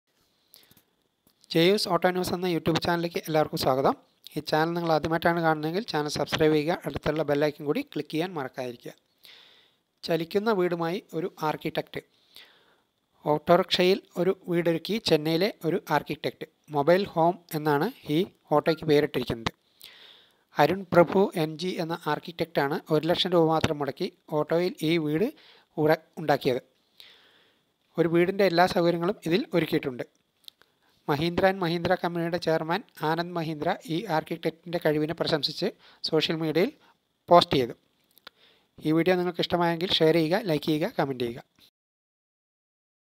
ചലിക്കുന്ന ഓട്ടോ വീട് #"mobile home "innovation chennai India Anand mahindra #
ഓട്ടോയിൽ ഒരു വീടൊരുക്കി അർച്ചിടെക്ട്. ചെന്നൈയിലെ അരുൺ പ്രഭു എന്ന ആളാണ് ഓട്ടോയിൽ ഒരു ചലിക്കുന്ന വീട് പണിതത്. ഒരു വീടിനുവേണ്ട എല്ലാ സൗകര്യങ്ങളും ഇതിൽ ഒരുക്കിയിട്ടുണ്ട് ഇദ്ദേഹം.